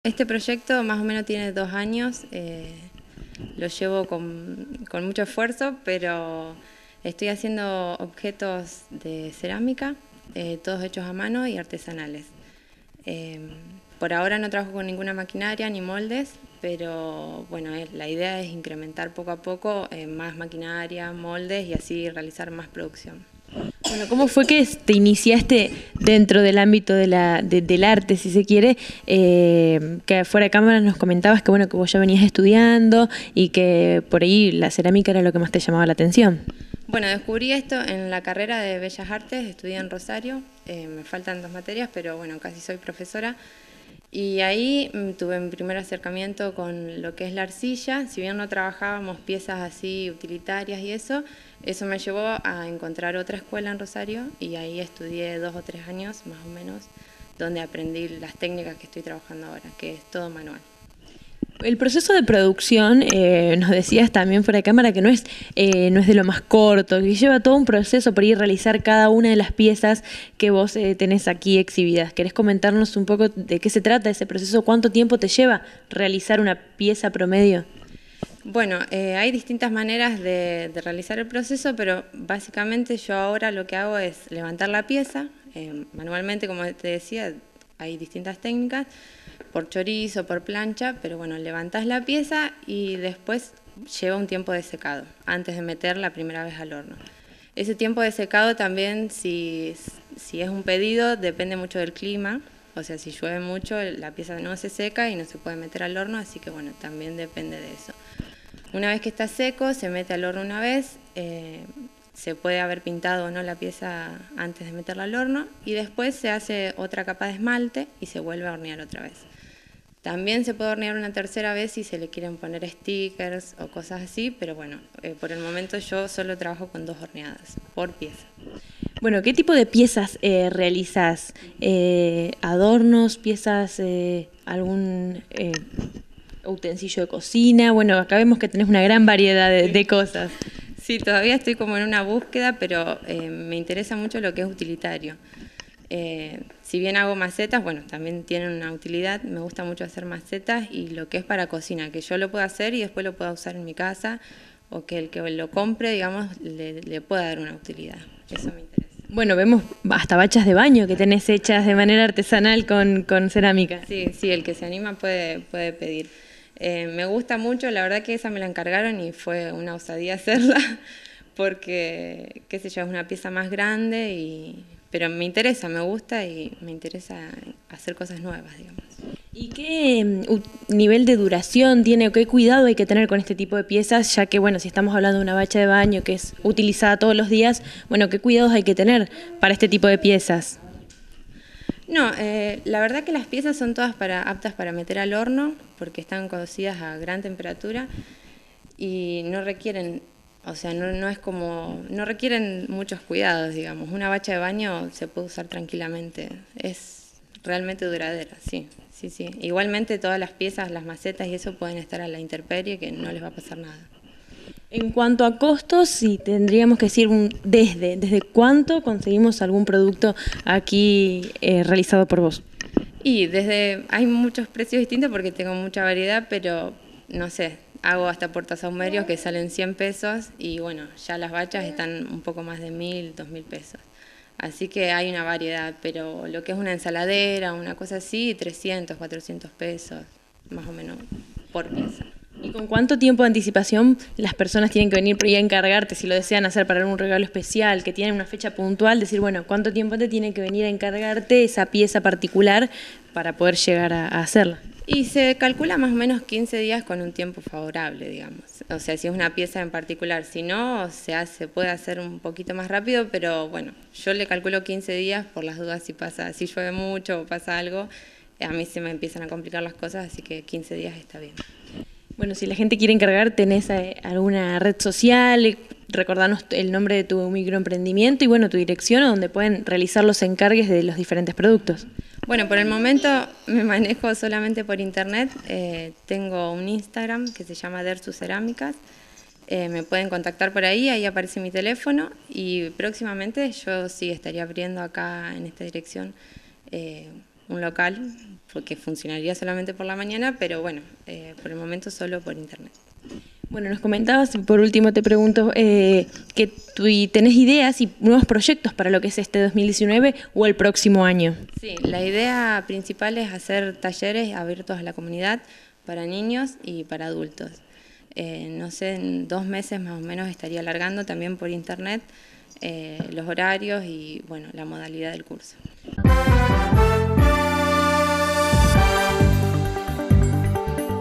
PAULA – DERSU CERÁMICA
MICROEMPRENDIMIENTO LOCAL REALIZA PRODUCTOS DE CERÁMICA ARTESANAL